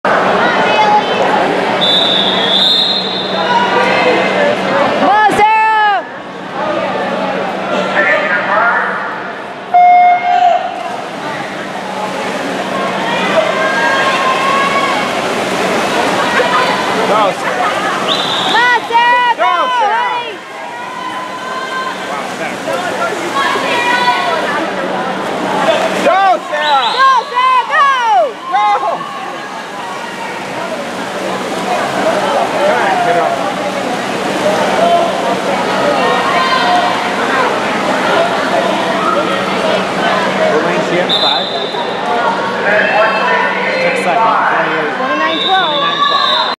Sarah Wilder 2013 50 Freestyle -- 12-29-13 Winter Whiteout Swim Meet Rochester NY
Sarah Wilder, age 13, competing in the 50 Yard Freestyle Event at the Winter Whiteout Invitational Meet in Rochester, NY at the Rochester Institute of Technology Pool on Sunday, December 29, 2013. Her final time was 29.12.  She placed 16th overall for the 13-14 group.